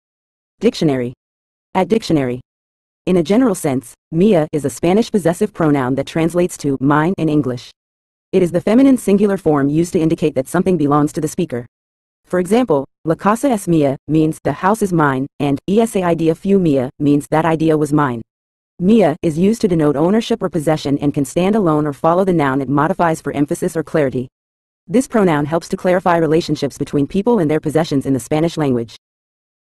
Mía Meaning In English
In a general sense, "mía" is a Spanish possessive pronoun that translates to "mine" in English. It is the feminine singular form used to indicate that something belongs to the speaker. For example, "La casa es mía" means "The house is mine," and "Esa idea fue mía" means "That idea was mine." "Mía" is used to denote ownership or possession and can stand alone or follow the noun it modifies for emphasis or clarity. This pronoun helps to clarify relationships between people and their possessions in the Spanish language.